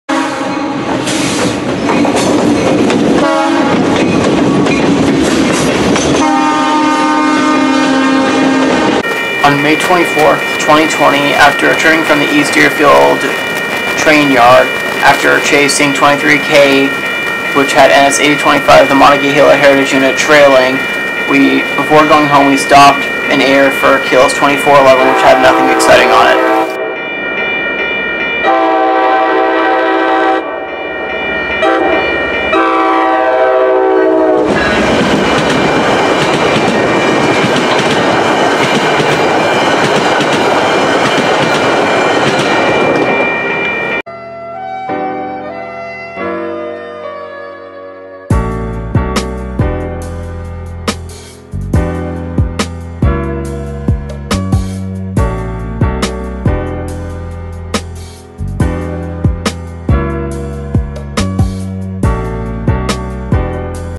MBTA Train in Ayer
On May 24th, 2020 after chasing 23K with NS 8025 befor going home we stopped at one of the crossings in Ayer for Keolis 2411 so enjoy.

0:00  Intro
0:08  Keolis 2411  (MBTX 2007  MBTX
0:44  Outro